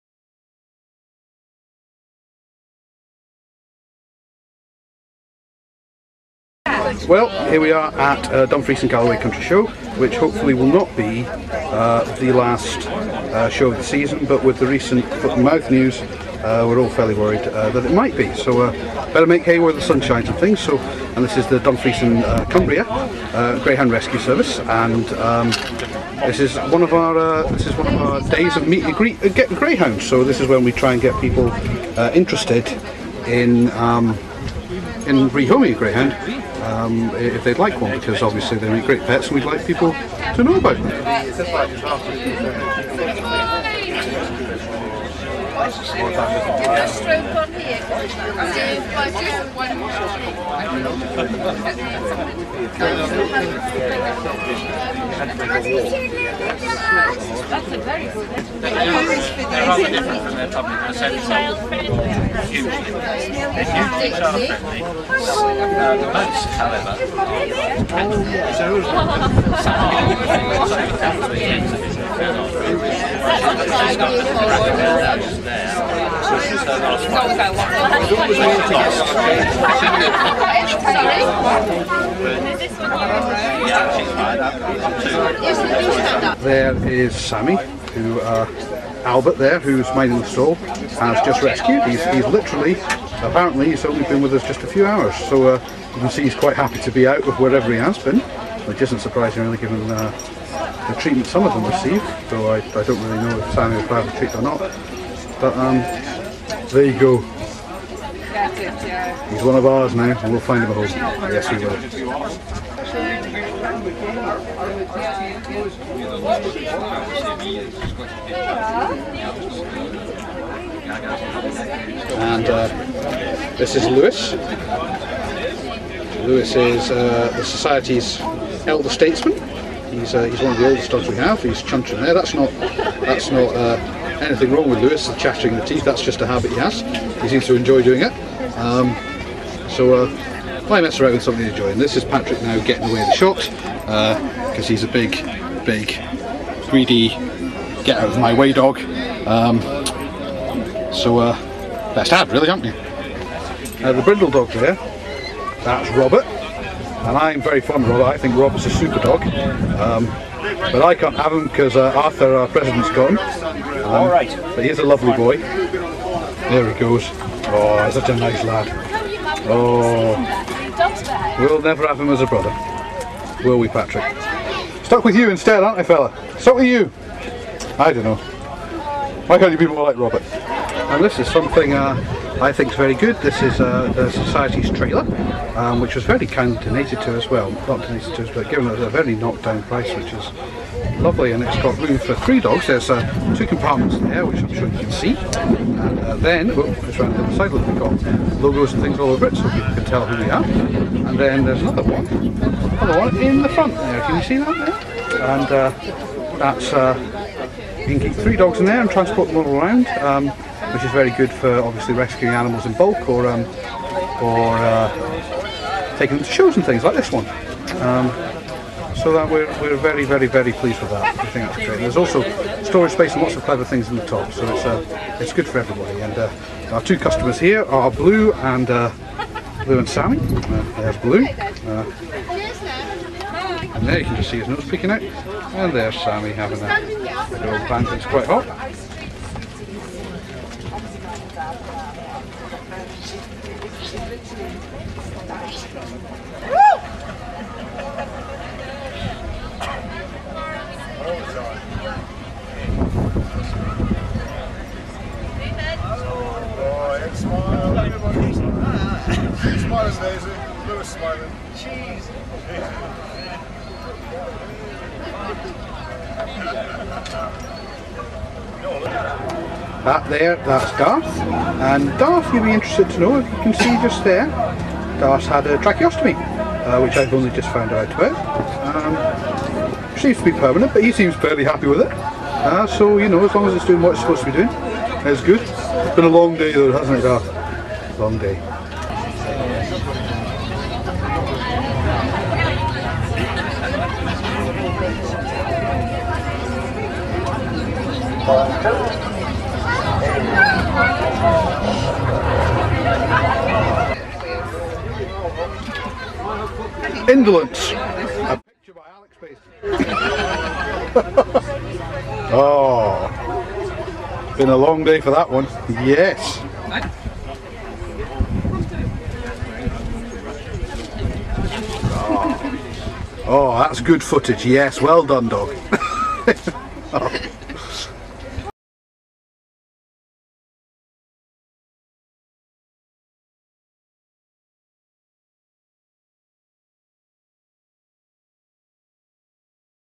Meet A Greyhound
A day at the Dumfries Country Show in Summer 2007 with Dumfries & Cumbria Greyhound Rescue, raising money and awareness of retired racing Greyhounds.